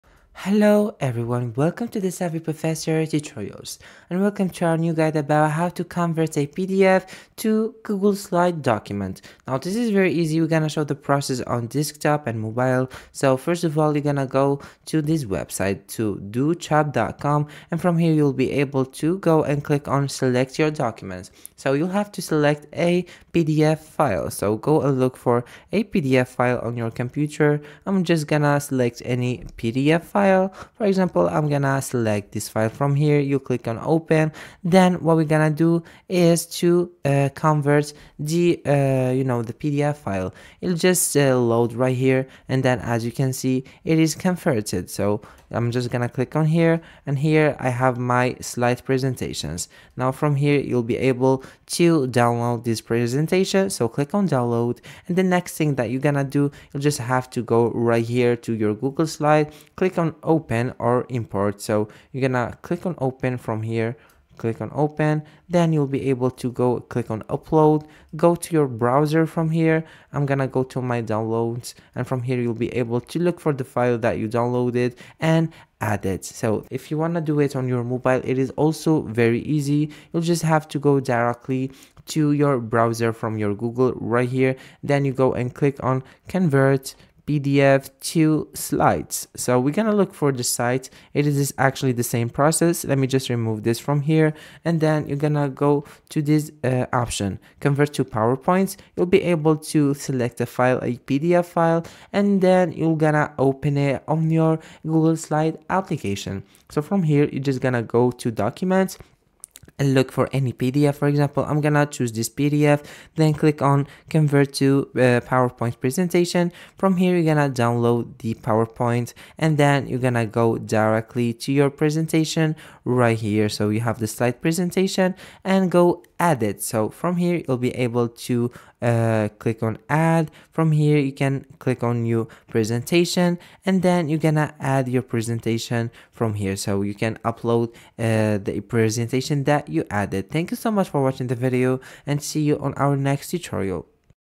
How To Convert PDF To Google Slides (Updated)
How To Convert PDF To Google Slides (Updated). In this video tutorial I will show how to convert PDF to Google Slides.